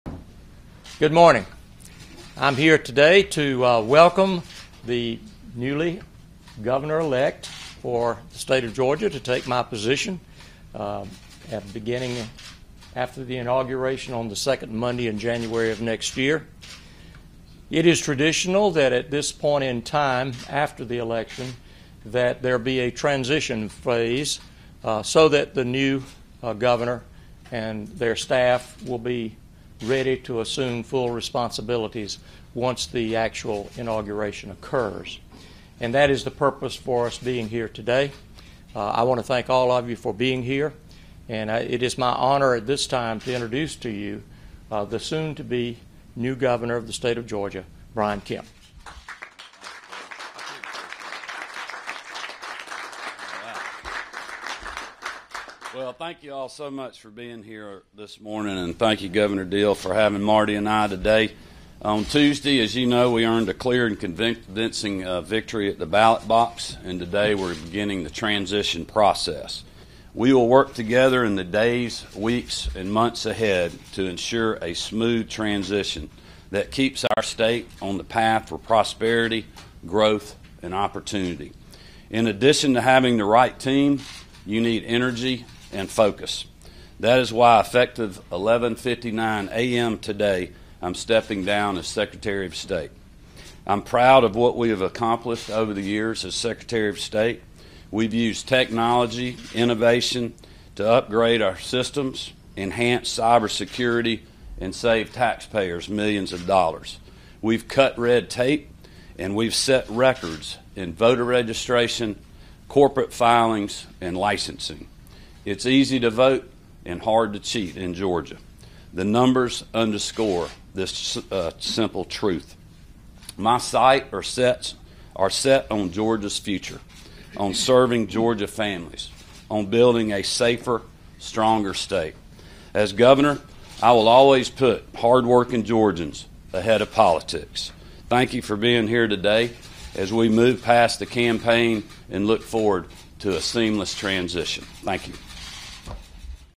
Kemp says he won Georgia governor race
(8 Nov 2018) Republican Brian Kemp resigned Thursday as Georgia's secretary of state, a day after his campaign said he's captured enough votes to become Georgia governor, even though his election rival is conceding nothing in one of the nation's marquee midterm races. (Nov. 8)